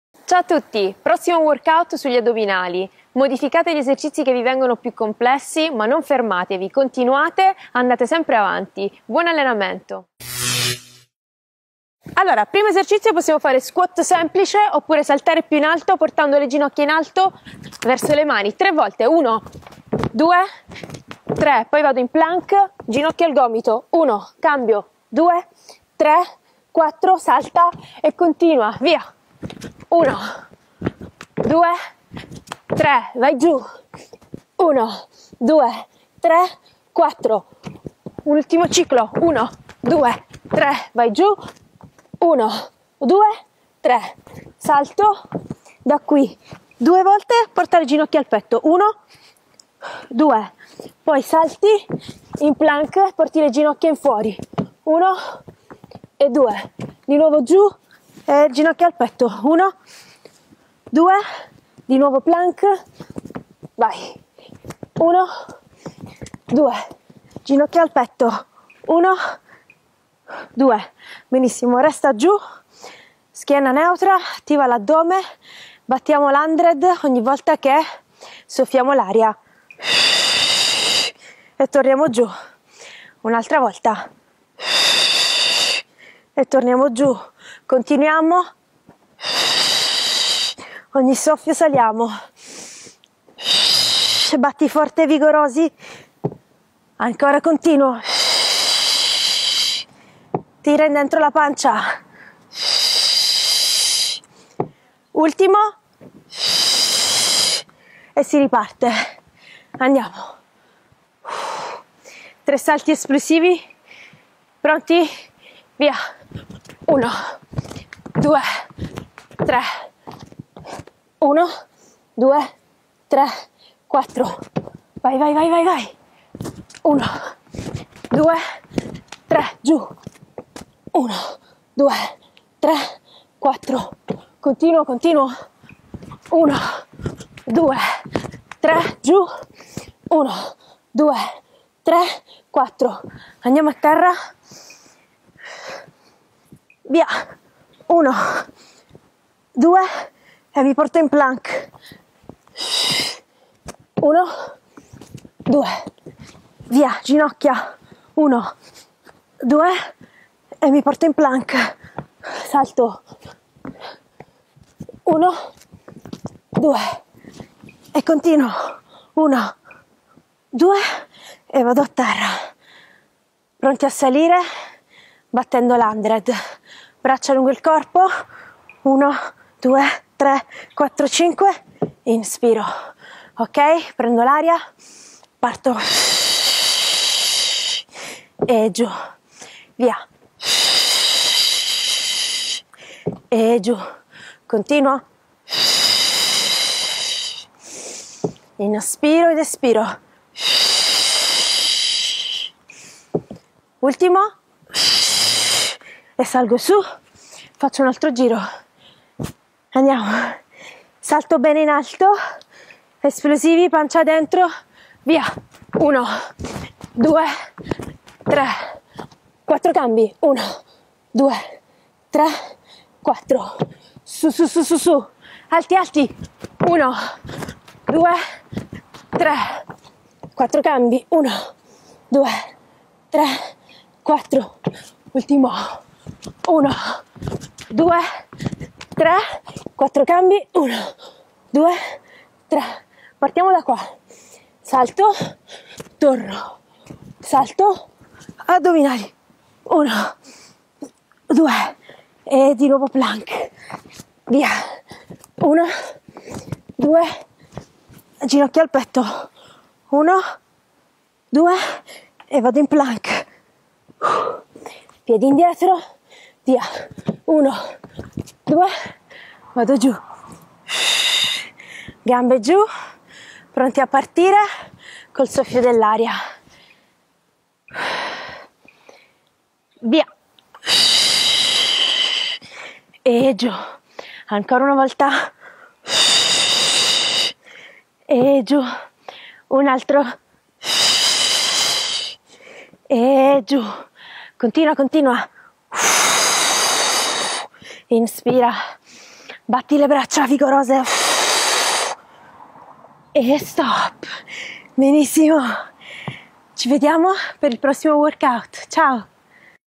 #30 QUARANTINE - workout pilates addominali - 5 minuti con Ines
Programma di allenamento di 5 minuti al giorno per 40 giorni consecutivi video #30
Un allenamento a corpo libero che rassoda tonifica e rinforza la muscolatura
Da fare tutti i giorni ed in maniera costante
Questo video è dedicato all'allenamento della parte superiore del corpo, ripetilo più volte o crea il workout componendolo con gli altri video della playlist